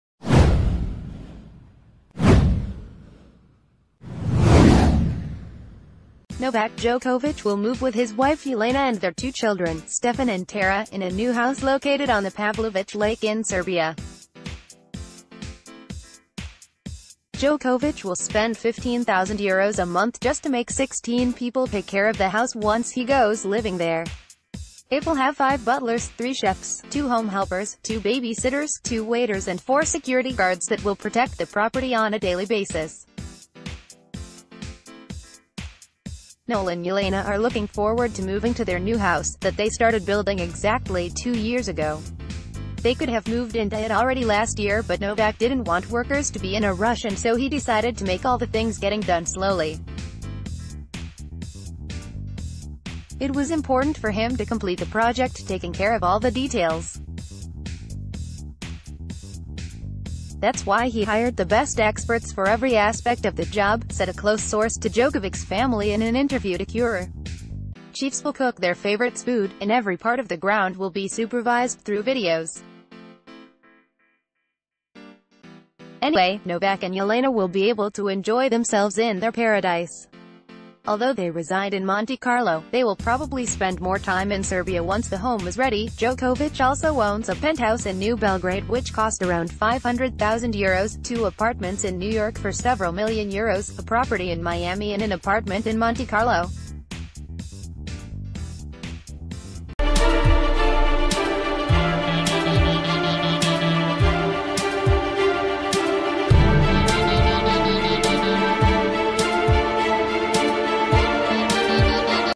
Novak Djokovic to move in stunning property in Serbia with family
Novak Djokovic will move with his wife Jelena and their two children, Stefan and Tara, in a new house located on the Pavlovic lake in Serbia. Djokovic will spend €15,000 a month just to make 16 people take care of the house once he goes living there. It will have five butlers, three chefs, two home helpers, two babysitters, two waiters and four security guards that will protect the property on a daily basis. 'Nole and Jelena are looking forward to moving to their new house, that they started bui...